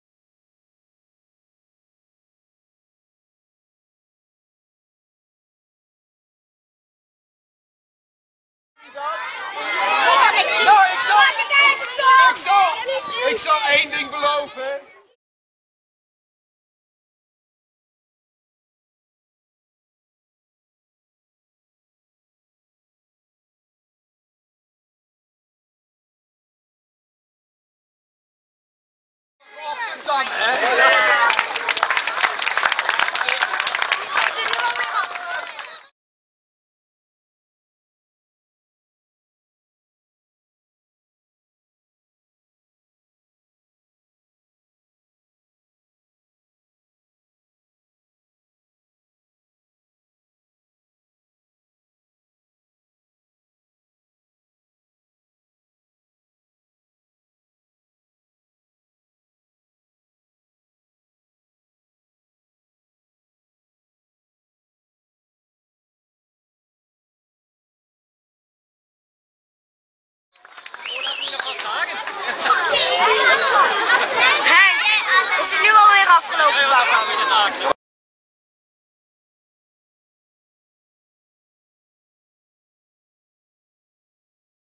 Bouwkamp Bottendaal 2004 t/m 2010
een overzicht van 7 jaar Bouwkamp Bottendaal 2004 t/m 2010.